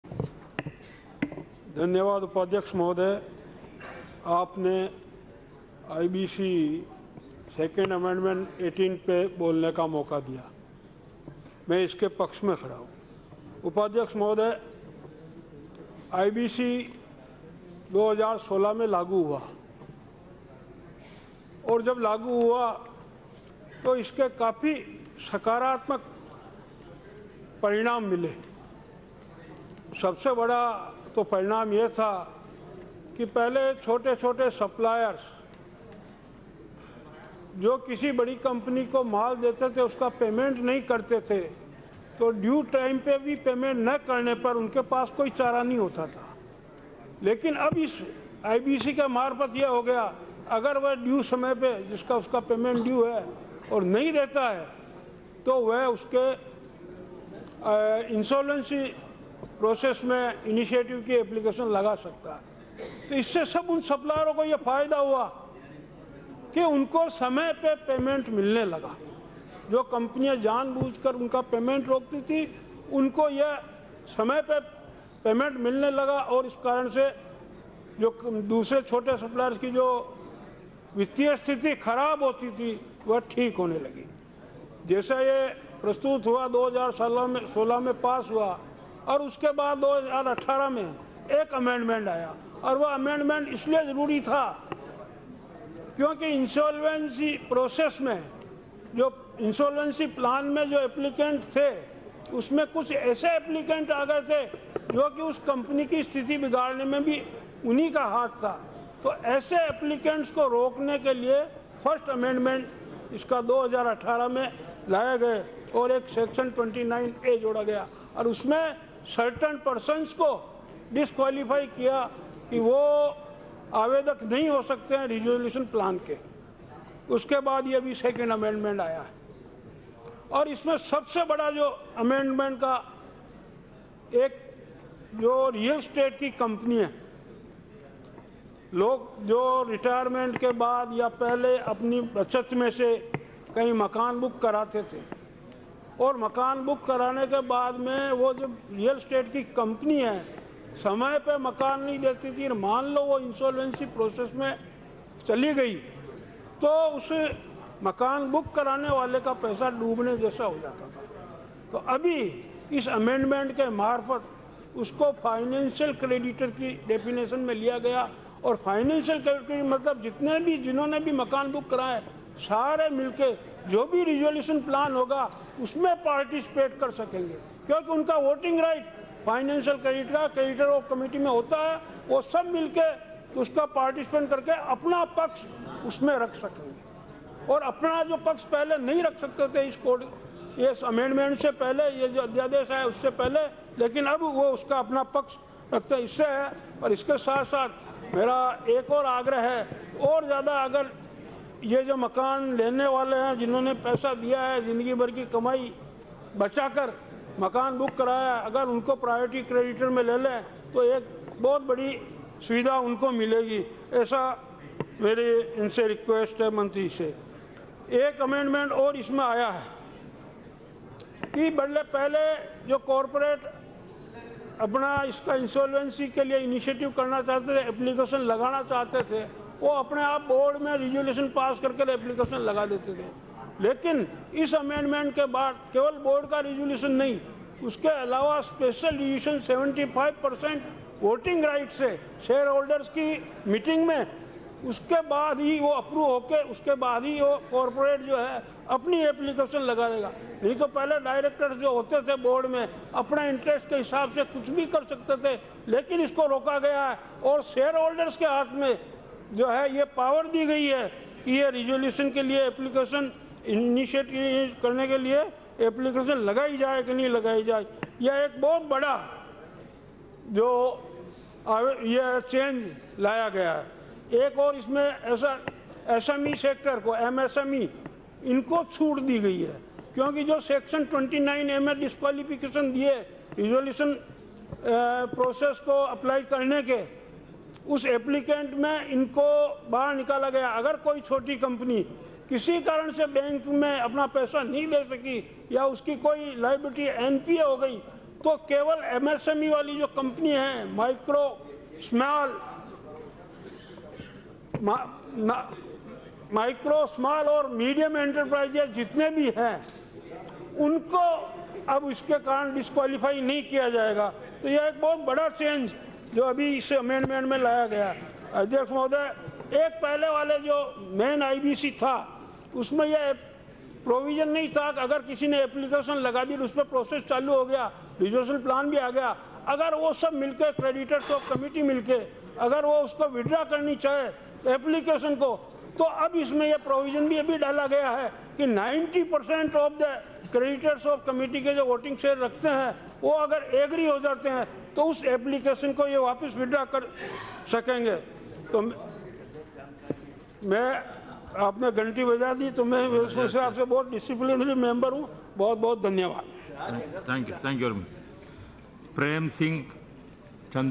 Subhash Chandra Baheria Talks About Insolvency and Bankruptcy Code | Parliament Session | NTV
Subhash Chandra Baheria Speaks on The Insolvency and Bankruptcy Code 2018 Introduced by Piyush Goyal  In Lok Sabha,  and the purpose of introducing  Insolvency and Bankruptcy Code bill in the Lok Sabha is to bring relief to the home buyers and Micro, Small and Medium Enterprises.  According to this Bill a person who buys land or building is considered as a financial creditor.